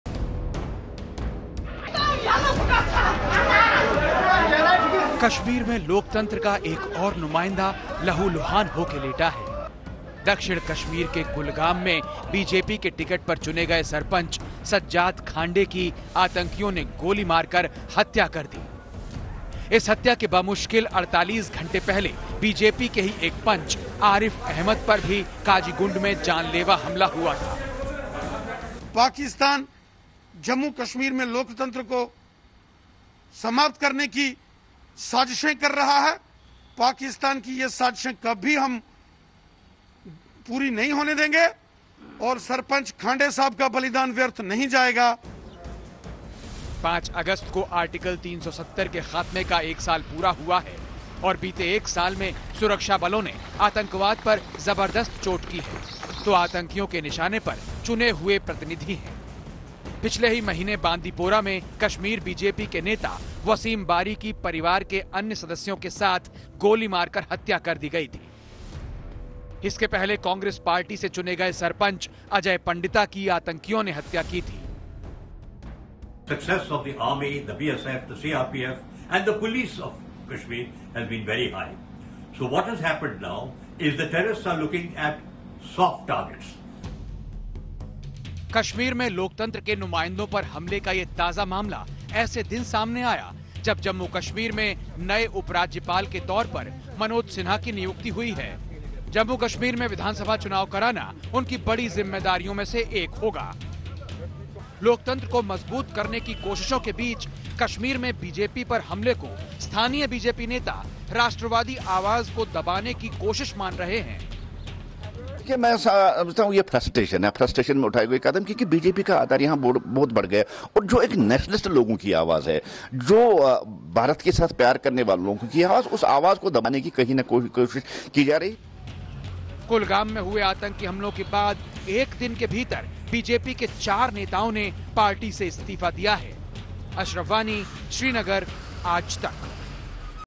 Jammu and Kashmir: सुरक्षाबलों से मिली हार तो लोकतंत्र पर निशाना? | Debate
5 अगस्त को आर्टिकल 370 के खात्मे का 1 साल पूरा हुआ है और बीते 1 साल में सुरक्षाबलों ने आतंकवाद पर जबरदस्त चोट की है तो आतंकियों के निशाने पर चुने हुए प्रतिनिधि हैं. इस बार दक्षिण कश्मीर के कुलगाम में BJP के टिकट पर चुने गए सरपंच सज्जाद खांडे की आतंकियों ने गोली मार के हत्या की. इस हत्या के बमुश्किल 48 घंटे पहले BJP के ही एक पंच आरिफ अहमद पर भी काजीगुंड में जानलेवा हमला हुआ था. तो आज कश्मीर में बढ़ रहे आतंकवाद पर होगी चर्चा.